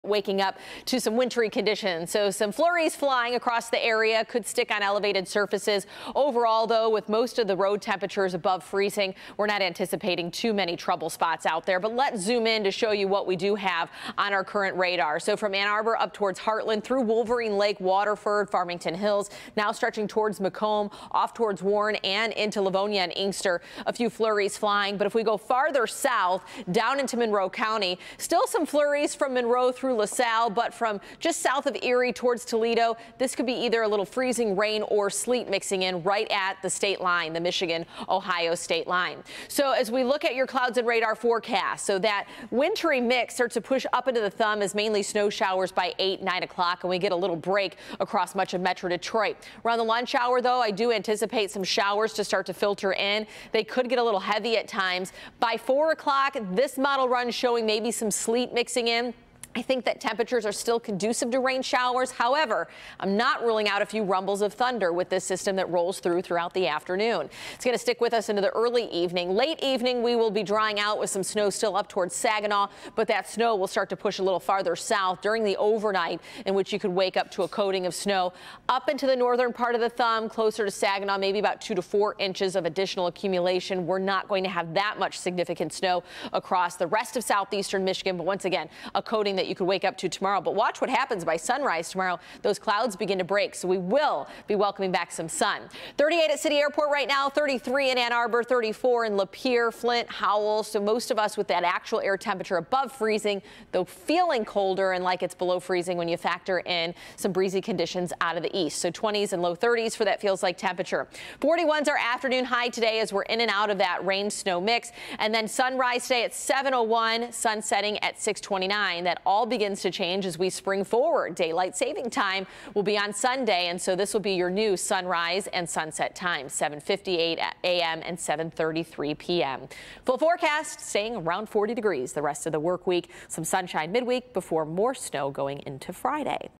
Tracking rain, snow Monday in Metro Detroit
We're tracking snow and rain crossing Southeast Michigan Monday morning, and in the afternoon in Metro Detroit. Here's what to expect.